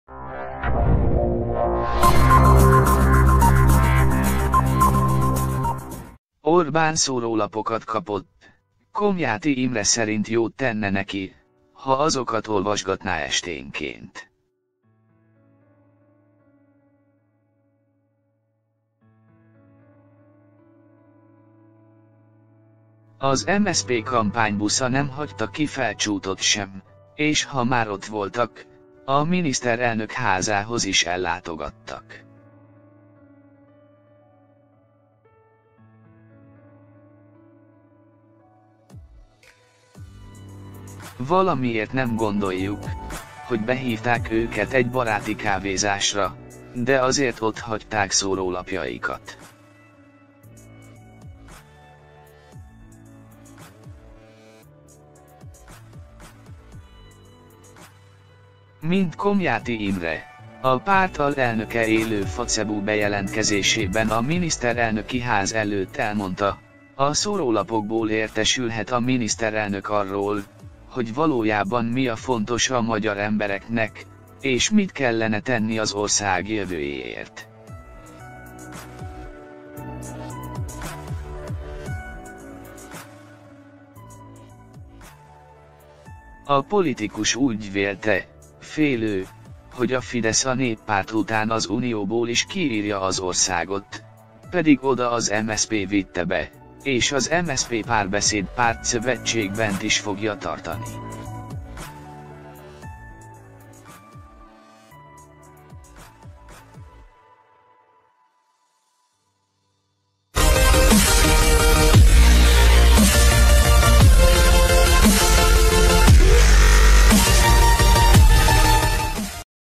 Nem azt találta Orbán a postaládában amire számított, pedig jobb lenne ha ezt is elolvasná!
Orbán szórólapokat kapott, Komjáthi Imre szerint jót tenne neki, ha azokat olvasgatná esténként.

Az MSZP kampánybusza nem hagyta ki Felcsútot sem, és ha már ott voltak, a miniszterelnök házához is ellátogattak. Valamiért nem gondoljuk, hogy behívták őket egy baráti kávézásra, de azért ott hagyták szórólapjaikat.

Mint Komjáthi Imre, a párt alelnöke élő Facebook-bejelentkezésében a miniszterelnöki ház előtt elmondta, a szórólapokból értesülhet a miniszterelnök arról, hogy valójában mi a fontos a magyar embereknek, és mit kellene tenni az ország jövőjéért.

A politikus úgy vélte, félő, hogy a Fidesz a néppárt után az unióból is kiírja az országot, pedig oda az MSZP vitte be, és az MSZP-Párbeszéd pártszövetség bent is fogja tartani.